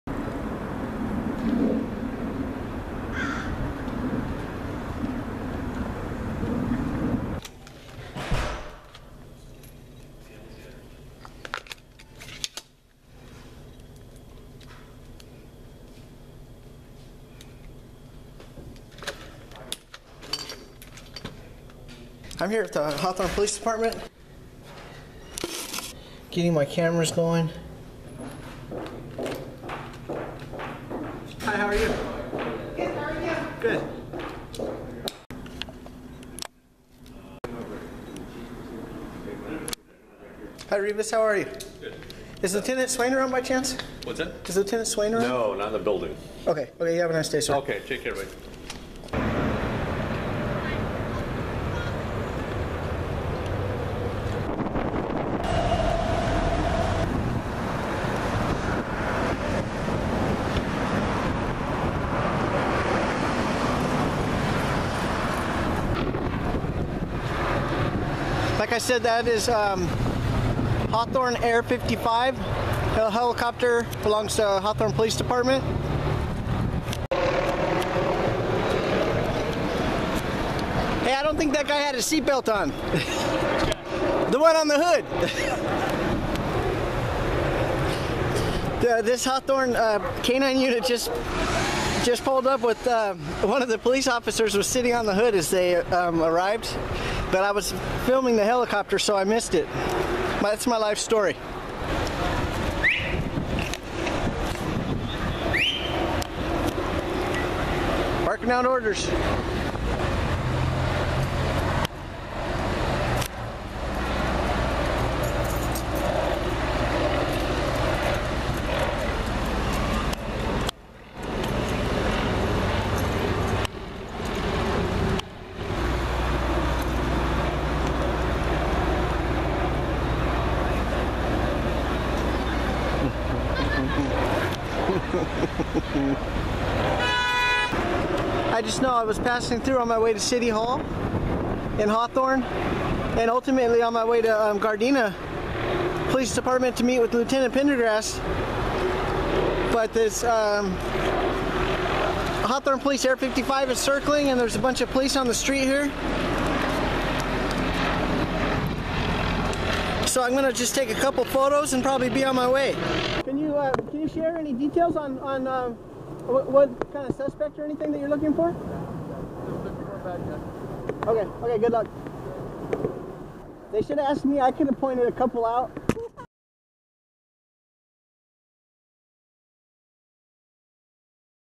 HAWTHORNE POLICE AIR 55 - LOOKING FOR A BAD GUY
Hawthorne Police helicopter, Air 55, is seen helping several cops as they "look for a bad guy."
As usual, lots of faces and cops you know by name if you frequent my channels.